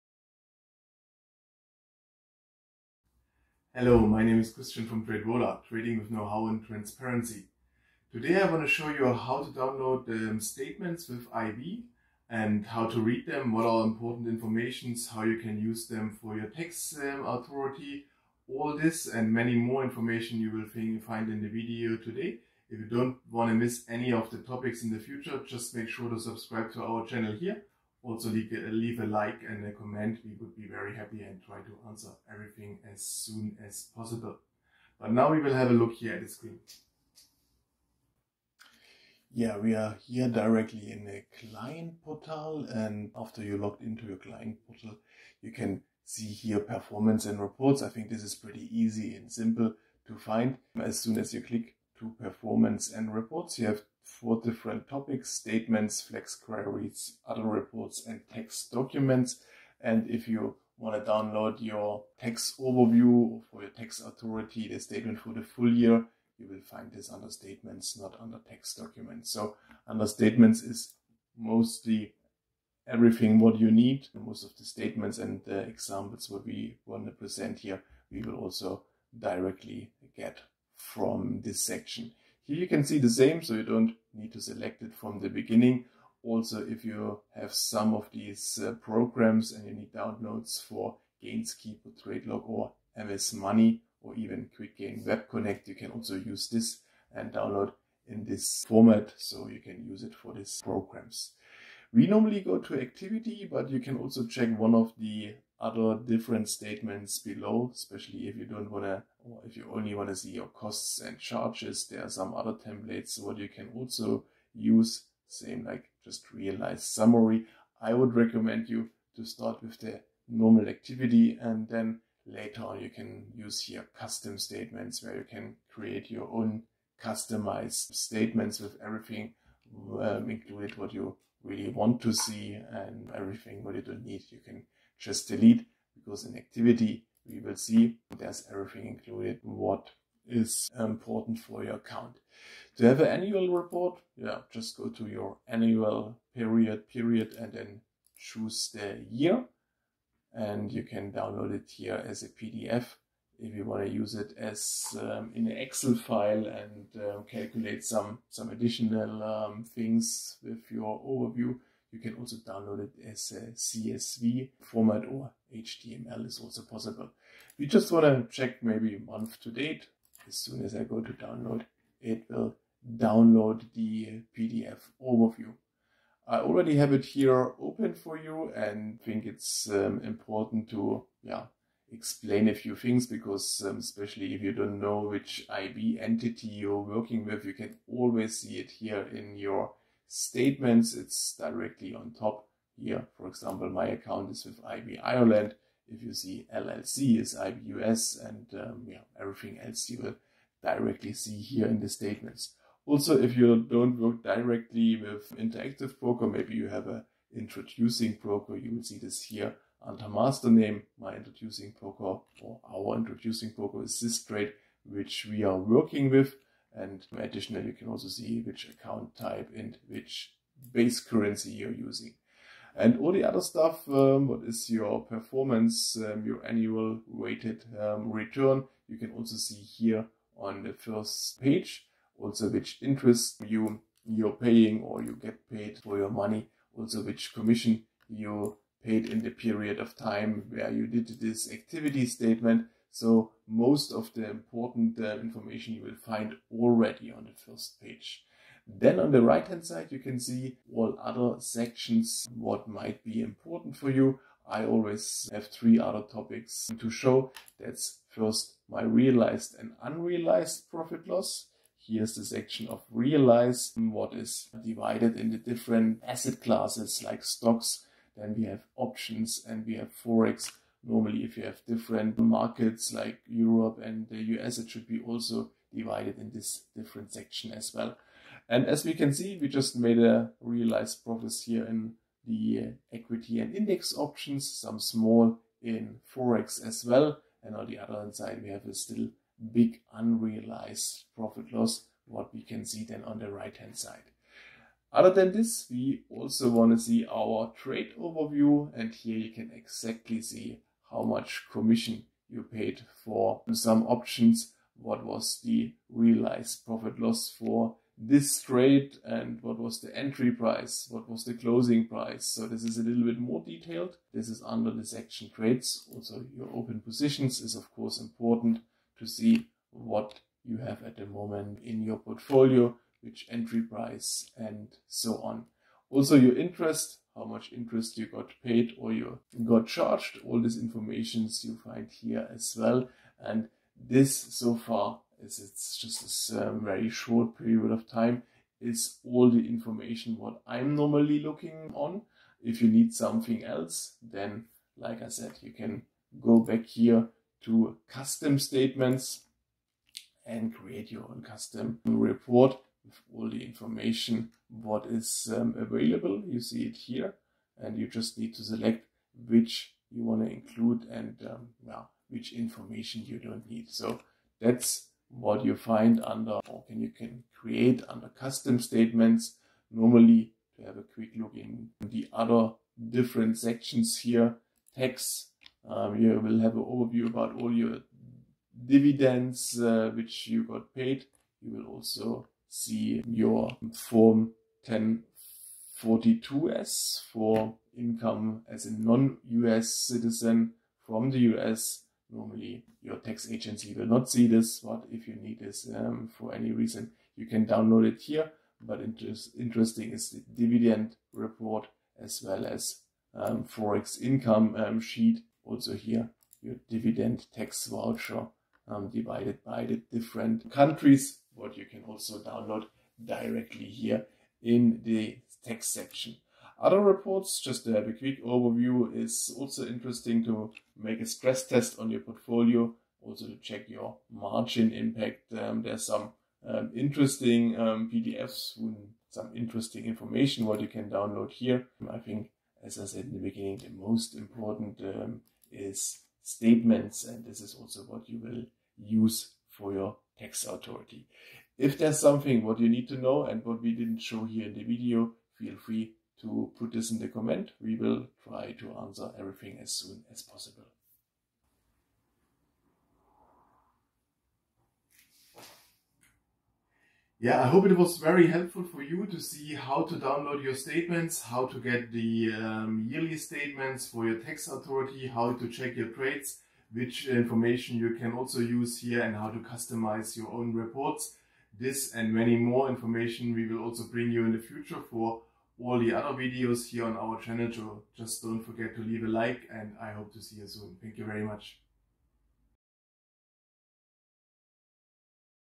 How to read and create statements and custom reports by Interactive Brokers
🔽Timestamps:
0:07 Intro
0:39 Client portal
2:18 Download Activity Statement
2:58 Explanation PDF Statement
5:47 Custom Statement
6:12 other Reports / Dividend Report
7:31 Summary